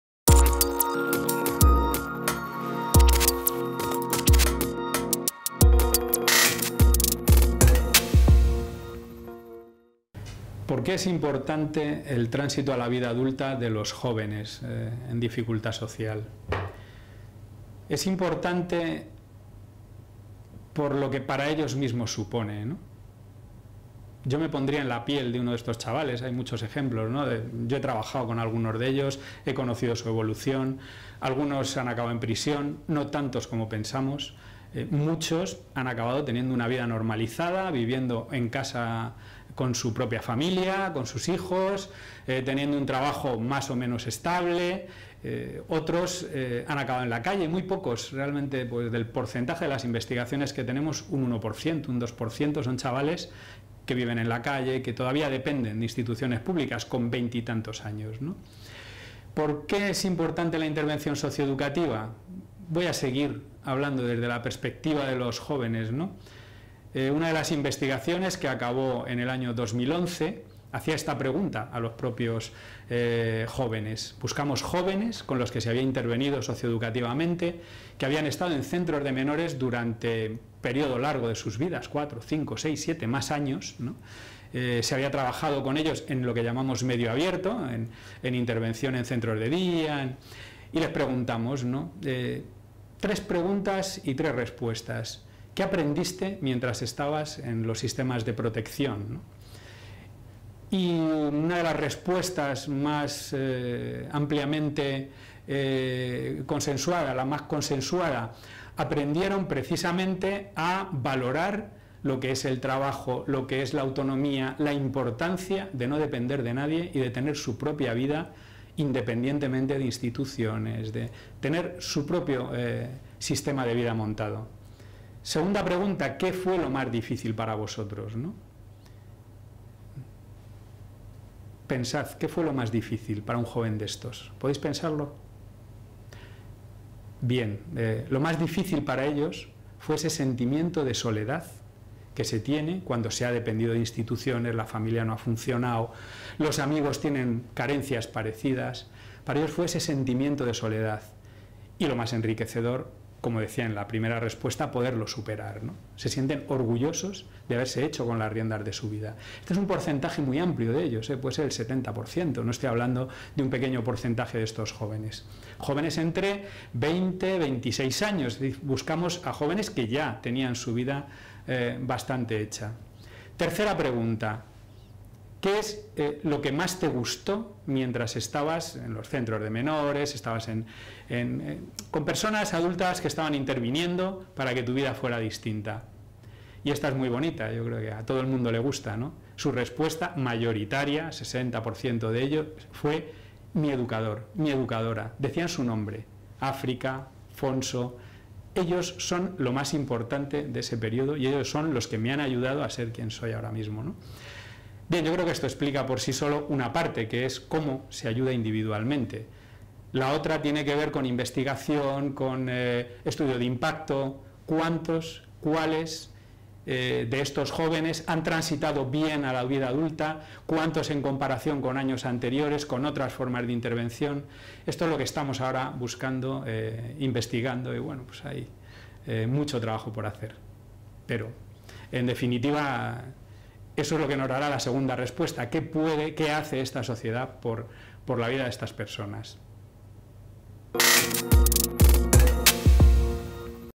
5.Tránsito a la vida adulta de jóvenes con necesidades educativas especiales Por qué es importante
El profesor Miguel Melendro Estefanía nos explica el tránsito a la vida adulta de jóvenes con necesidades educativas especiales. ¿Por qué es importante la intervención socioeducativa sobre el tránsito a la vida adulta de los jóvenes en dificultad social.

Miguel Melendro Estefanía
Profesor del Departamento de Teoría de la Educación y Pedagogía Social, UNED

Titulo del ciclo
Especialización en intervención sociocultural para contextos escolares y no escolares/Especialização em Intervenção Sociocultural para contextos escolares e não-escolares.

Para saber más:
UNED

Producción: Carlos Busón Buesa, CEMAV- UNED
Realización: José Luís De la Calle Muñoz, CEMAV - UNED